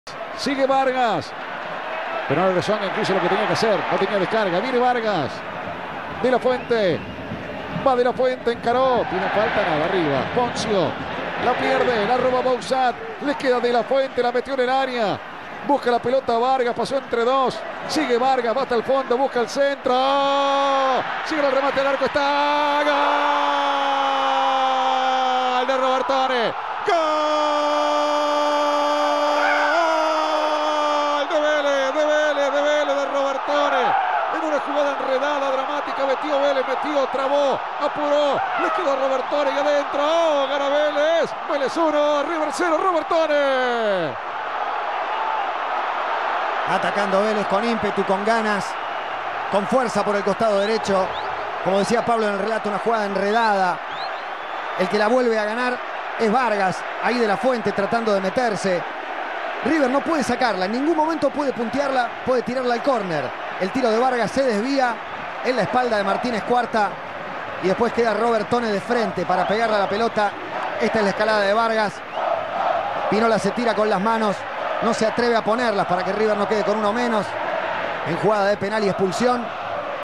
El gol de Lucas Robertone a River
Reviví el golazo de Robertone para certificar el triunfo ante River.